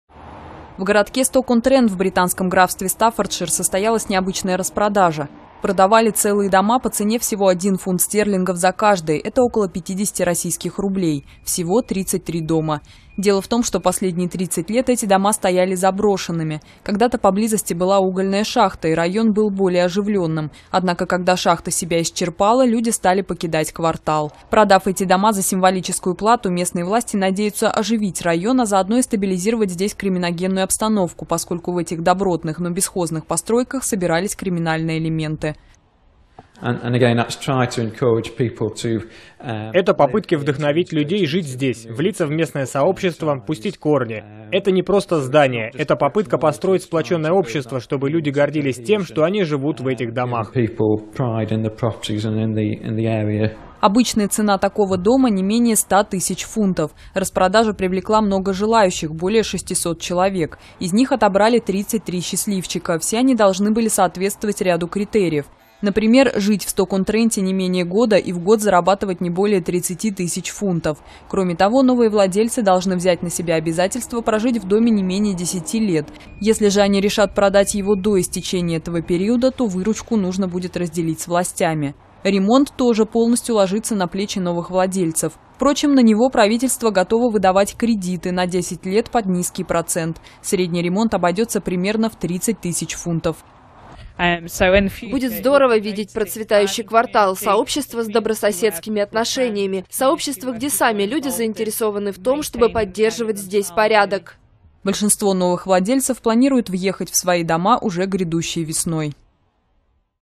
Купить дом за 1 фунт можно было в Великобритании (новости)
Купить дом за 1 фунт можно было в Великобритании. В городке Сток-он-Трент в британском графстве Стаффордшир состоялась необычная распродажа. Продавали целые дома по цене один фунт стерлингов за каждый, это около 50 российских рублей. Всего -- 33 дома.

Дело в том, что последние 30 лет эти дома стояли заброшенными. Когда-то поблизости была угольная шахта, и район был более оживленным. Однако когда шахта себя исчерпала, люди стали покидать квартал.

Продав эти дома за символическую плату, местные власти надеются оживить район, а заодно и стабилизировать здесь криминогенную обстановку, поскольку в этих добротных, но бесхозных постройках собирались криминальные элементы.

[Мэтью Никсон, сотрудник городского органа самоуправления]:
«Это - попытки вдохновить людей жить здесь, влиться в местное сообщество, пустить корни. Это не просто - здания, это - попытка построить сплоченное сообщество, чтобы люди гордились тем, что они живут в этих домах».

Обычная цена такого дома -- не менее 100 тысяч фунтов. Распродажа привлекла много желающих -- более 600 человек. Из них отобрали 33 счастливчика. Все они должны были соответствовать ряду критериев. Например, жить в Сток-он-Тренте не менее года и в год зарабатывать не более 30 тысяч фунтов. Кроме того, новые владельцы должны взять на себя обязательство прожить в доме не менее 10 лет. Если же они решат продать его до истечения этого периода, то выручку нужно будет разделить с властями.

Ремонт тоже полностью ложится на плечи новых владельцев. Впрочем, на него правительство готово выдавать кредиты на десять лет под низкий процент. Средний ремонт обойдется, примерно, в 30 тысяч фунтов.

[Рэйчел Робертс, покупательница дома]:
«Будет здорово видеть процветающий квартал, сообщество с добрососедскими отношениями, сообщество, где сами люди заинтересованы в том, чтобы поддерживать здесь порядок».

Большинство новых владельцев планируют въехать в свои дома уже грядущей весной.